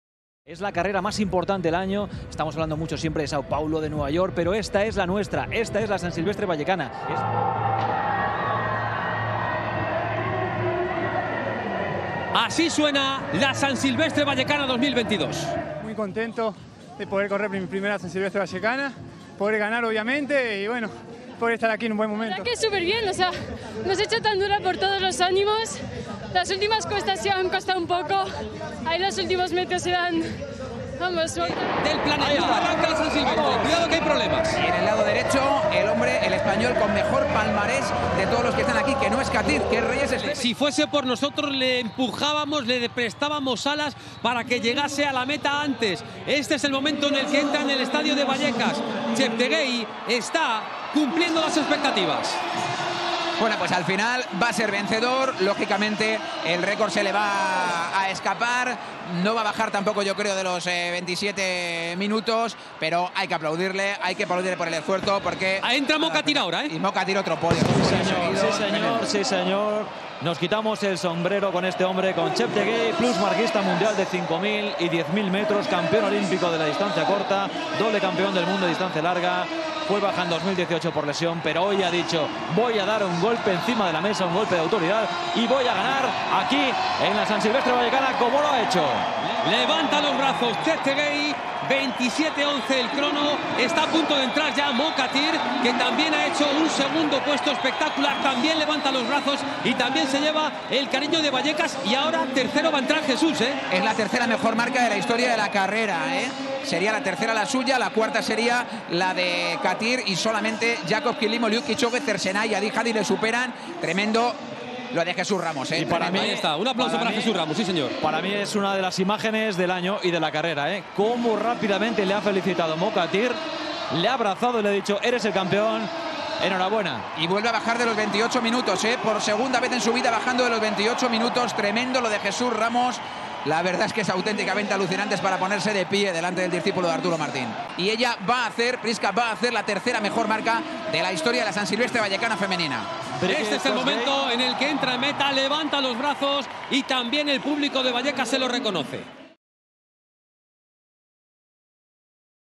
Los mejores momentos de la San Silvestre Vallecana 2022
El atleta español Mohamed Katir no pudo defender victoria en la San Silvestre Vallecana 2022 al ceder este sábado ante el ugandés Joshua Cheptegei, mientras que la también ugandesa Prisca Chesang se impuso en la carrera femenina.